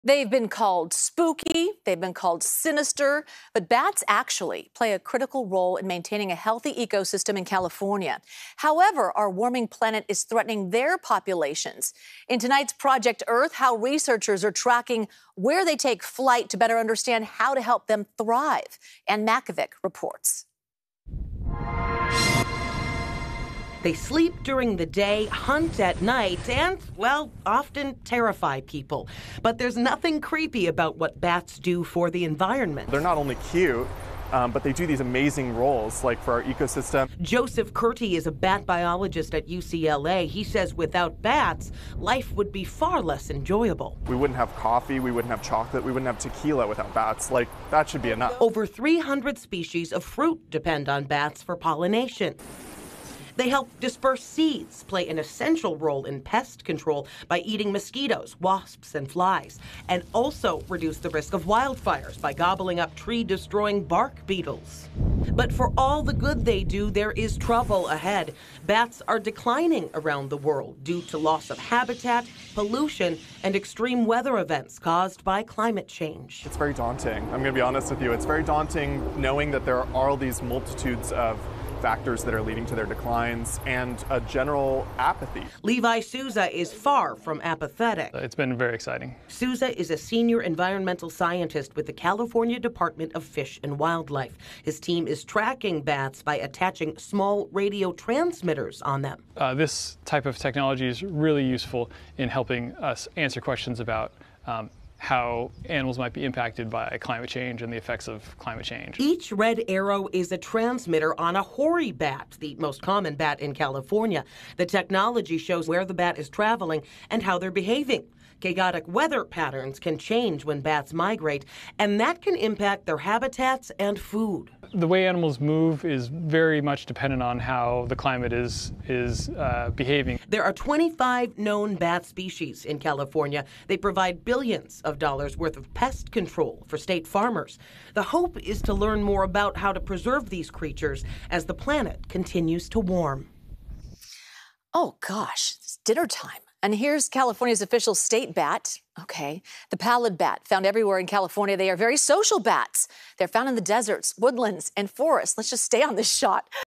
Scientists track valuable bats for their protection in a warming world
Bats play a critical role in maintaining California's ecosystem as well as the state's valuable agriculture industry. Anne Makovec reports.